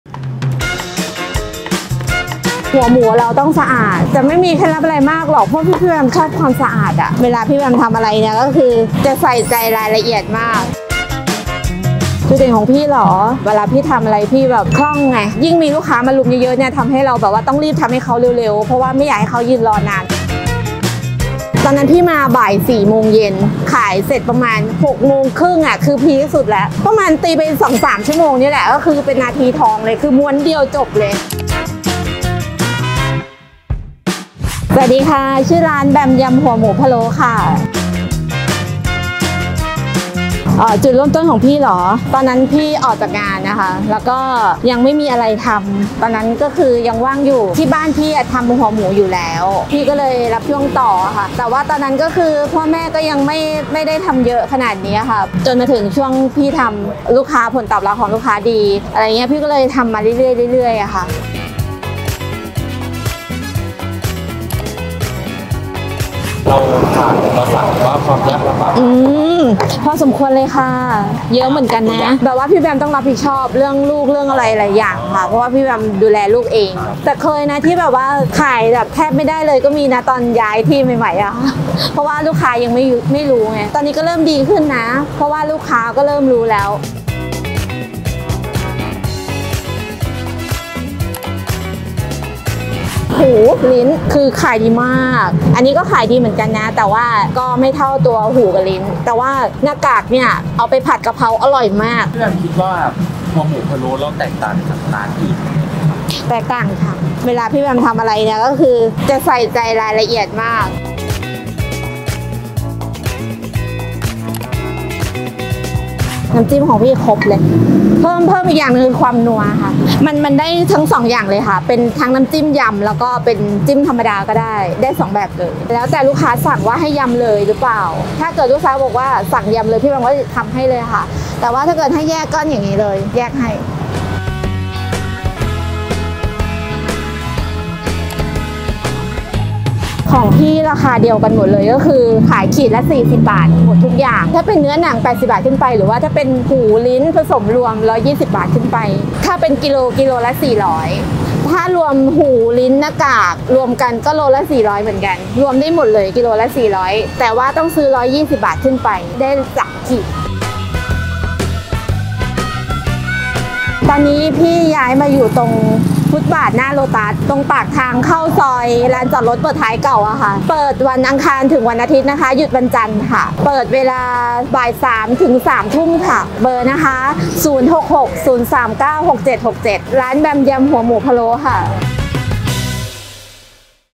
ยำหัวหมูพะโล้ คุณแม่เลี้ยงเดี่ยว !!มือสับขั้นเทพ ขาย 2 ชั่วโมงหมด
ยำหัวหมูพะโล้ คุณแม่เลี้ยงเดี่ยว !!
มือสับขั้นเทพ ขาย 2 ชั่วโมงหมด
TidReviewINSIDE วันนี้เจ๊พามาอยู่ที่ร้าน “เจ๊แบม ยำหัวหมูพะโล้” จากคุณแม่เลี้ยงเดี่ยวขายยำหมูพะโล้ส่งลูกเรียนจนจบ เป็นร้านหัวหมูพะโล้ รสแซ่บ คนรุมขายดีมากจนต้องรับบัตรคิวเลย ถึงจะย้านที่ขายแต่ก็ยังขายดี และขายดีทุกอย่างไม่ว่าจะเป็นลิ้นหมู เนื้อหมู หน้ากากหมู อยากได้แบบไหนเลือกเอาตามใจชอบ ขายดีชนิดที่ว่าขาย 2 ชั่วโมงหมดดด แถมน้ำพะโล้ น้ำยำสูตรเด็ดจากคุณแม่ ทำเอง ปรุงเอง วัตถุดิบที่นี่คือคัดเกรดอย่างดี สด ใหม่ทุกวัน!! ทุกอย่างราคาขีดละ 40 บาท เท่านั้นเองเธอออ
น้ำยำรสแซ่บ จัดจ้าน ครบรส เปรี้ยว หวาน เค็ม หัวหมูนุ่ม เปื่อยกำลังดี หอมเครื่องพะโล้มาก ลิ้นหมูจะมีความหนึบๆ กลิ่นหอม รสชาติกลมกล่อม ส่วนหน้ากากหมู มันจะนุ่มไปอีก เวลากินคู่กับน้ำจิ้มก็คือเข้ารสกันสุดๆ ที่ร้านจะใช้หมูที่ไม่มันมาก ที่สำคัญคือทำสะอาด ล้างเอย ปรุงเอย กลิ่นหอม ไม่เหม็นสาป ปรุงคู่กับเครื่องพะโส้สูตรเด็ดยิ่งหอมเข้าไปอีก เรียกได้ว่าใส่ใจคุณภาพมากจริงๆค่ะ ร้านคุณแม่ปัจจุบันตั้งอยู่ที่ ฟุตบาทหน้าโลตัส ตรงปากทางเข้าซอย ลานจอดรถเปิดท้ายเก่า  เลยน้าาาา
ชื่อร้าน :  แบมหัวหมูพะโล้
พิกัด : ฟุตบาทหน้าโลตัส ตรงปากทางเข้าซอย ลานจอดรถเปิดท้ายเก่า
เวลาเปิด-ปิด : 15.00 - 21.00 น.